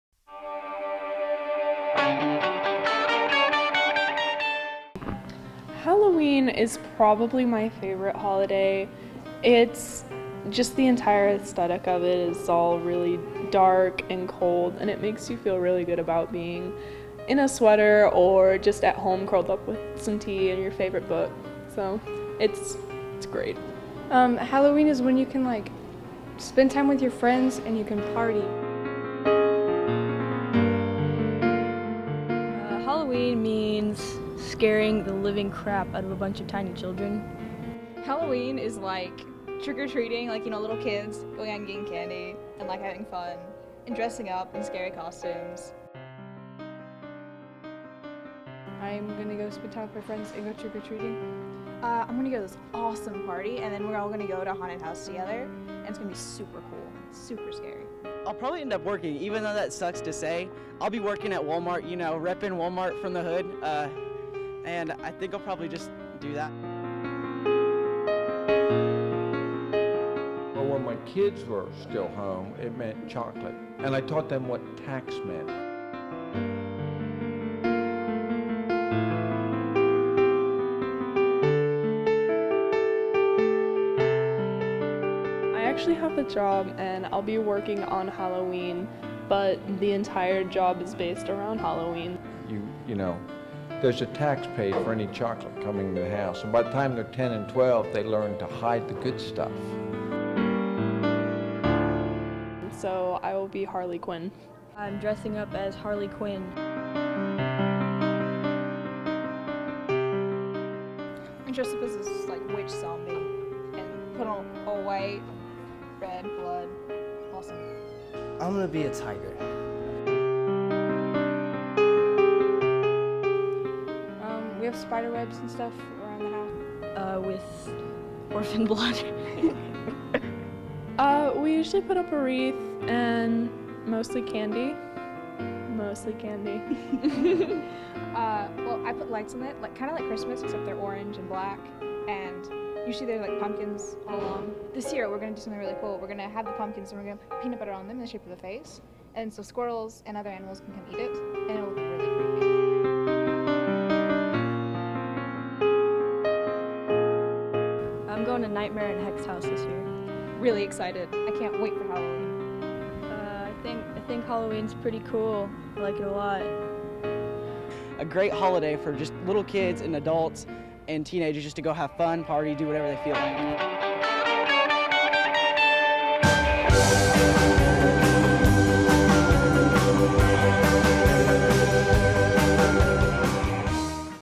AV Student Documentary: “Halloween.”
This student produced documentary by Bri Regnier, Sydney Brown, and Taylor Lanning is about how Halloween is celebrated by various students at BAHS and what people enjoy most about it.